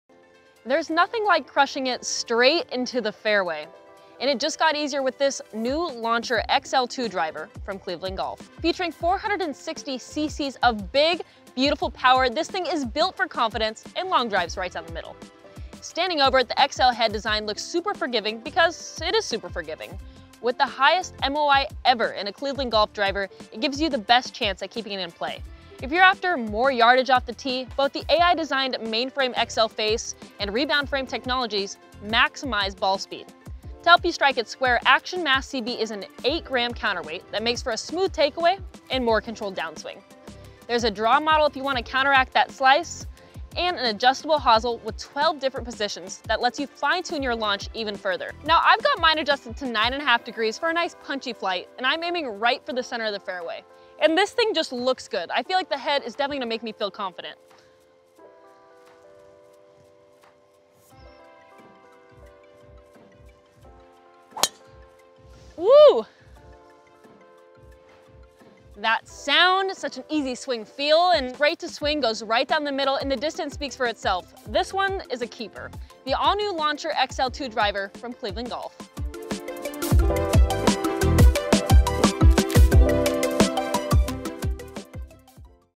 Cleveland Launcher XL 2 Drivers - BIG, BEAUTIFUL POWER
Cleveland Launch XL2 Driver

WINNER - 2024 Golf Digest Hot List Gold Medal

It's BIG and built for one thing: sending bombs down the middle. The all-new Launcher XL 2 Draw Driver delivers more distance, forgiveness, and consistency than ever thanks to new MainFrame technology, an improved XL design, and more. And since XL 2 Draw helps compensate for your pesky slice, all you gotta do is step up and swing BIG..